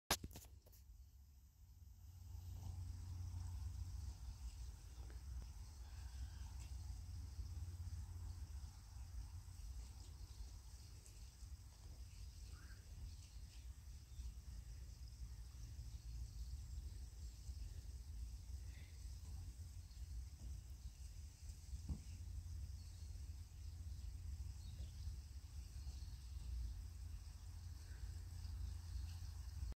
Deer's grazing on grass
While I was up in the hills I saw a full family of Deer grazing on the grass